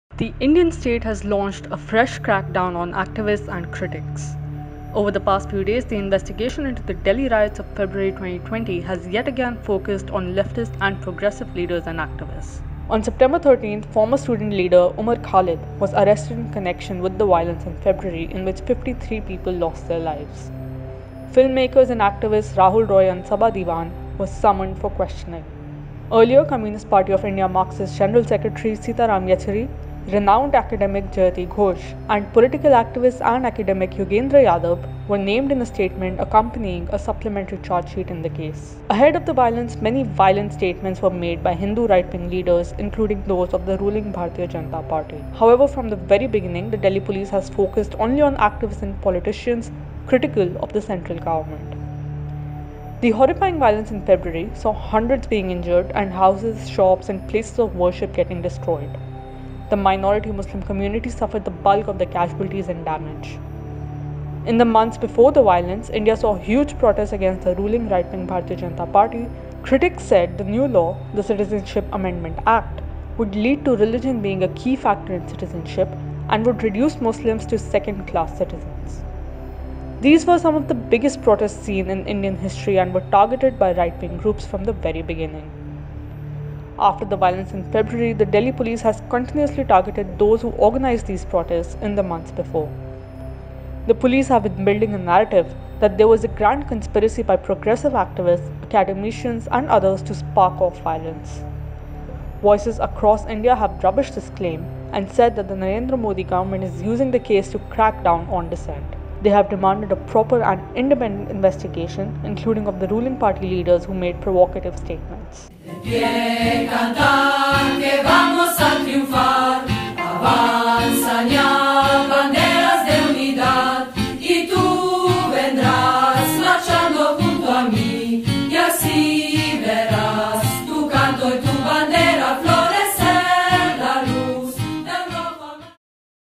India continues crackdown on activists, critics of govt.
The investigation into the Delhi riots of February 2020, which led to the death of 53 people, has from the very beginning focused on critics of the government and activists who had organized anti-government protests earlier. On the other hand, right-wing leaders, including those of the ruling BJP, who made provocative speeches just ahead of the violence continue to roam free. Over the past few days, the government has moved against several leading leftists and progressive political and social activists in this case.